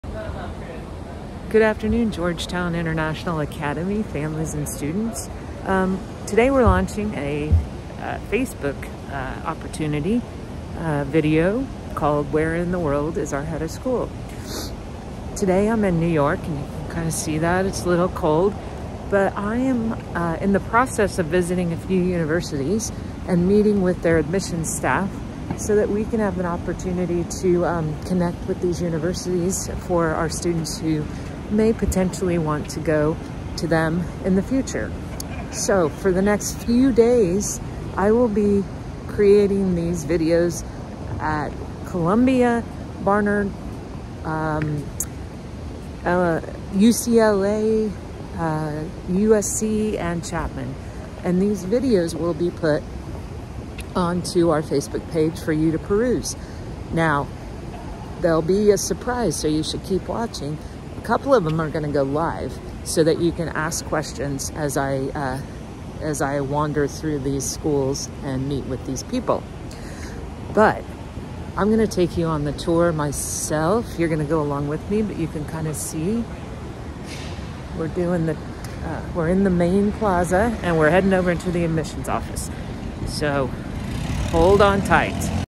Where In The World Is Our Head of School, Exploring NYU: Episode 1
Where in the World is our Head of School - Episode 1: Exploring NYU

🎥 Welcome to the first episode of "Where in the World is our Head of School"! Join us on this exciting journey as our Head of School explores various universities, sharing incredible opportunities that could shape our students' futures.

🗽 Episode 1: Exploring NYU in New York City
In this episode, we delve into the heart of New York City to explore New York University (NYU). Discover the vibrant campus, the diverse academic programs, and the unique experiences that NYU offers to students. Our Head of School provides insights into the admission process, campus life, and the city's rich cultural backdrop.

🔍 Highlights:

Campus Tour: Walk through the iconic NYU campus, showcasing key facilities and student hangout spots.
Academic Insights: Learn about the range of academic programs and opportunities available to students.
Student Life: Explore the dynamic student life at NYU and the cultural experiences the city provides.
👀 Watch Now: Where in the World is our Head of School - Episode 1

Join us as we unravel the possibilities that lie ahead for our students in the world of higher education.